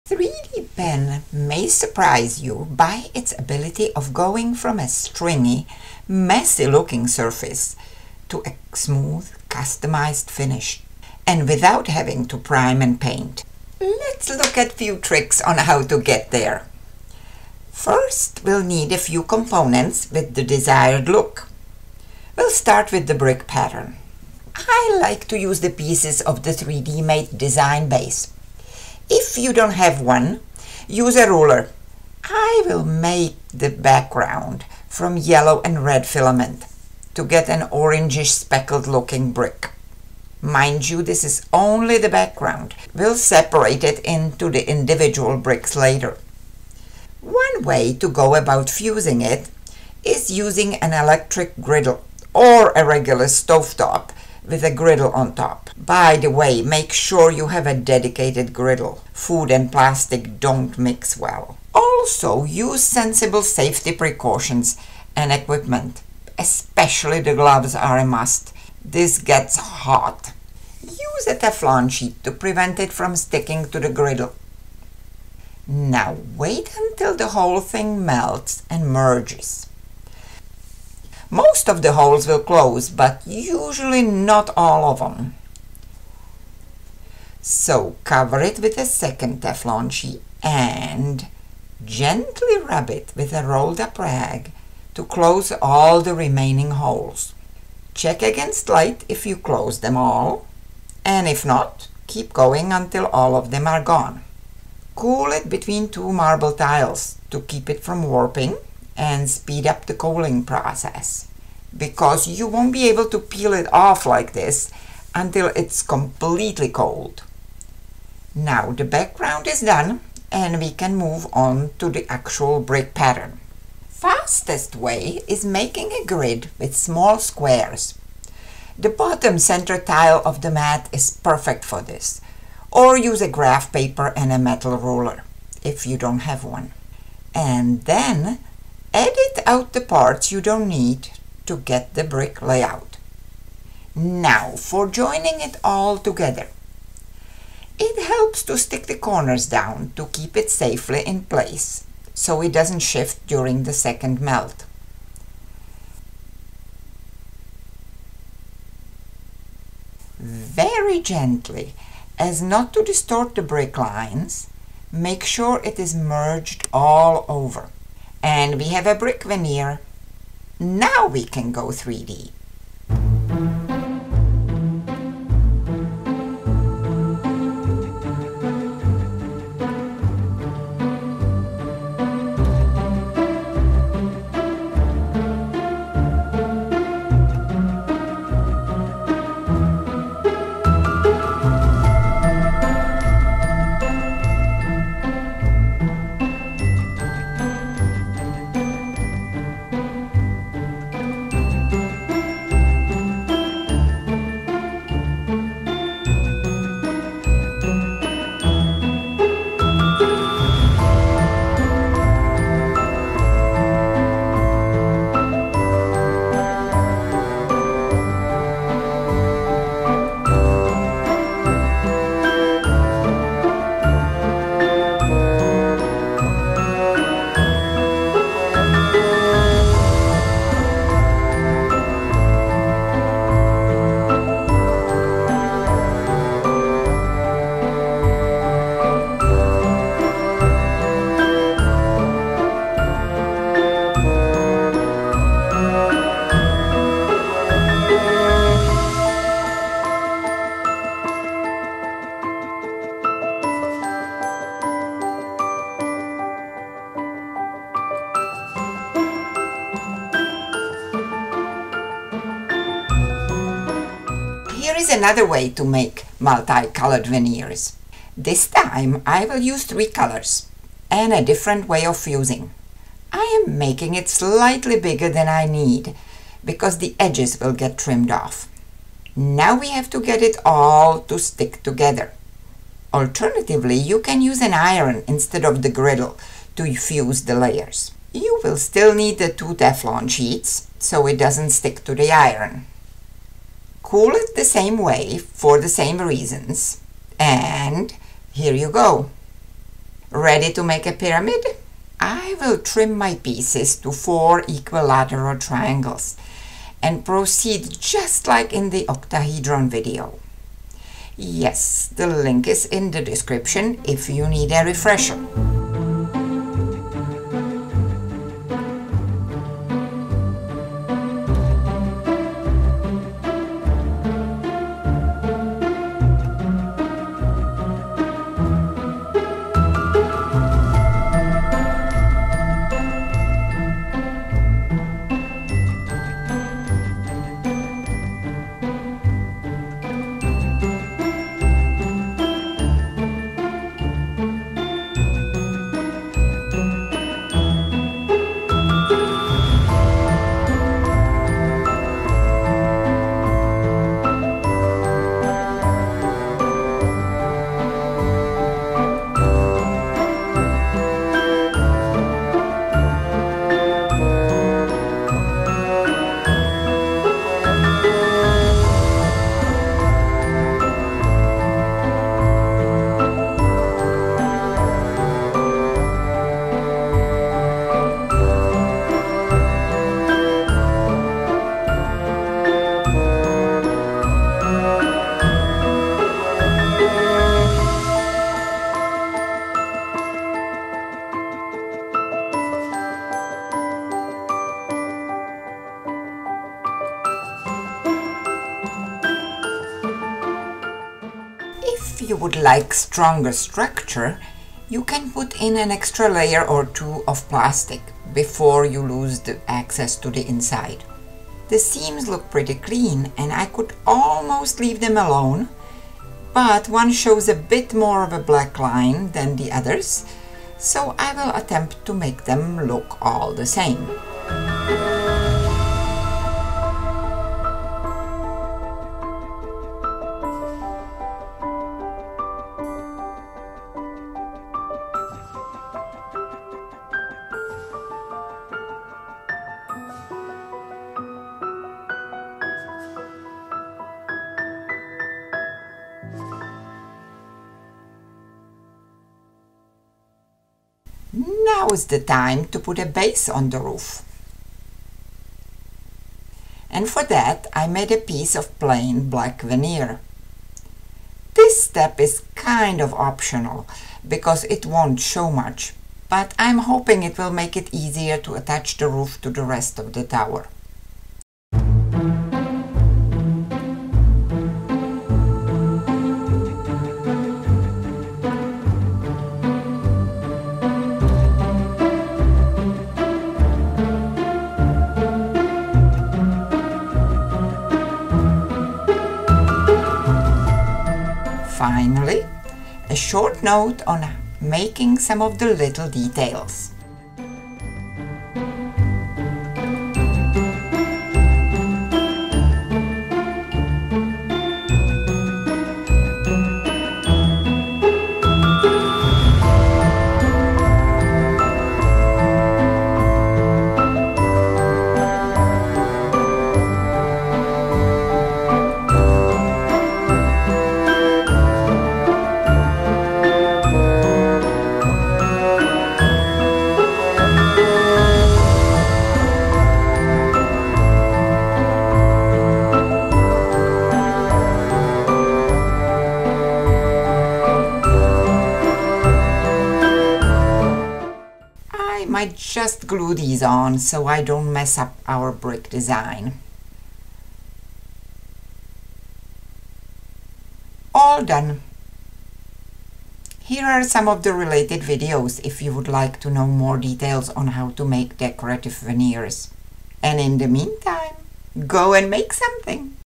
Customized 3d pen surfaces
Custom-made multicolored surfaces elevate your 3d pen projects to the next level. This video demystifies the process of making and using complex 3d pen generated veneers. Great information for artists, STEM teachers, art teachers, homeschooling parents, crafters, and all 3D pen enthusiasts.

CONTENTS:
0:00 Making custom surfaces
0:22 Brick veneer
2:54 Tower assembly
4:26 Roof veneer
5:00 Roof assembly
8:12 Doors & windows

TOOLS AND MATERIALS:
3d pen patterns

Mat: 3Dmate Base - Transparent 3D Pen Mat 18 x 12 Inches

3d pen

PLA Filament:
OVERTURE PLA
HATCHBOX PLA

Working surface:
Plexiglass 12” x 12” Square Panel 1/8” Thick (3mm)
KRYLON Matte Finish Spray

Heat tools:
Heat gun with adjustable temperature and heat flow
Heat tools for smoothing and soldering:
Welding Soldering Iron with Thermostatic Digital-Controlled and LCD Screen Display
Walnut Hollow wood-burning tool
Replacement tips set 1
Replacement tips set 2

Other useful tools:
Teflon sheets for ironing, baking, and embossing 3 Pack
Cooling tile marble 12x12
Wire/filament cutter pliers
Fiskars scissors
Fiskars sharpener